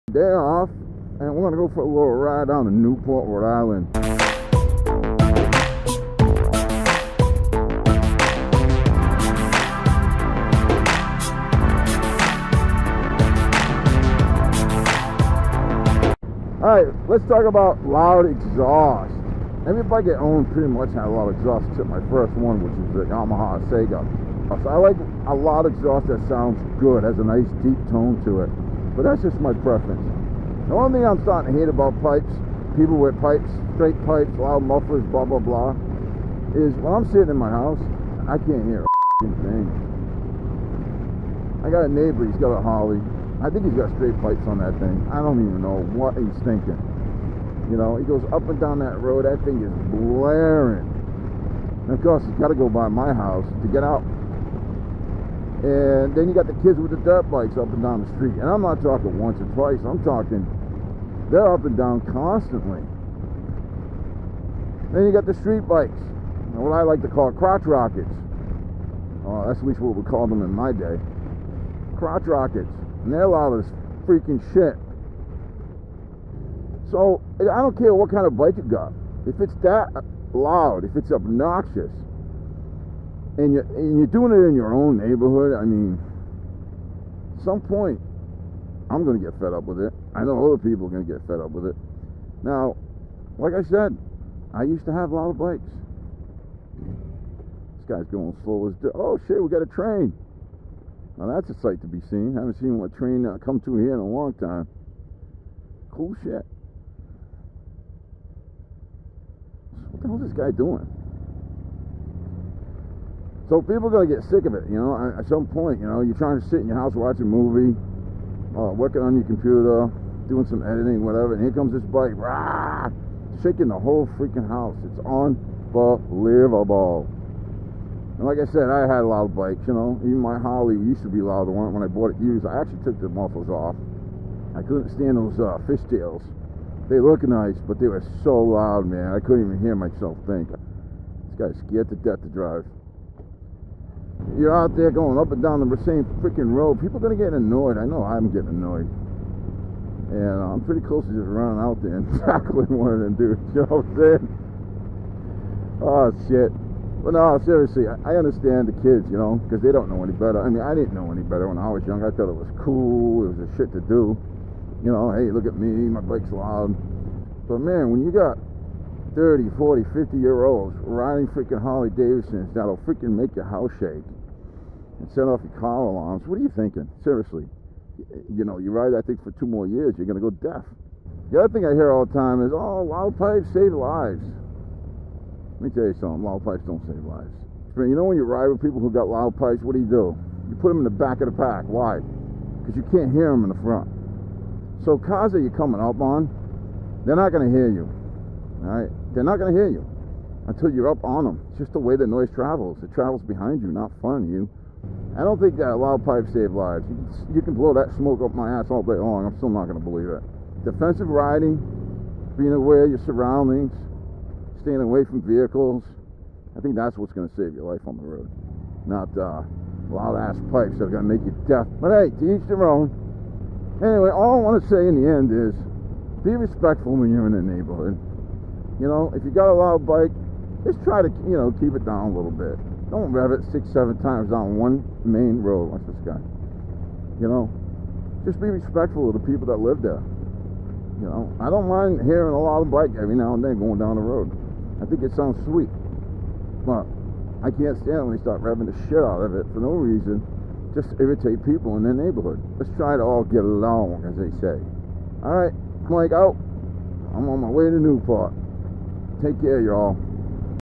Loud Motorcycle Pipes Don't Save Lives-It Annoys People | Techn' moto
Loud Motorcycle Pipes Don't Save Lives- it Annoys Folks- Freestyle Thursdays #15

Another Freestyle Thurdays Motovlog! I get into talking about loud pipes, if they are effective in saving lives or if they just annoy everyday folks like you and me.

My MotoVlogging Equipment Setup-
GoPro Hero 5
GoPro Hero
Feiyu G5 3 Axis Handheld Gimbal
Sony ECMCS3 Mic
Furry Microphone Windscreen Muff